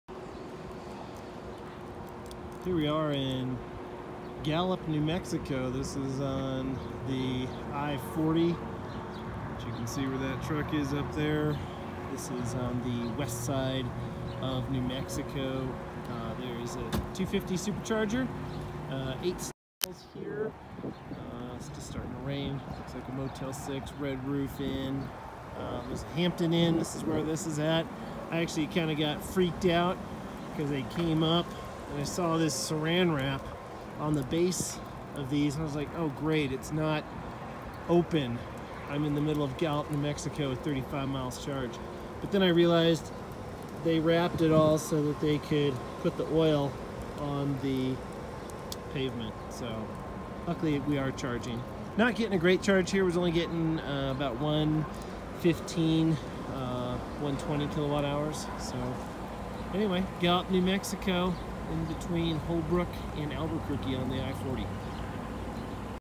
Supercharger Short: Gallup NM. 1100 Twin Buttes Rd
Quick video looking around the supercharger off I 40 between  between Albuquerque, NM and Flagstaff, AZ in Gallup NM.  Located in a Hampton Inn parking lot.  250 Kw, 8 stalls.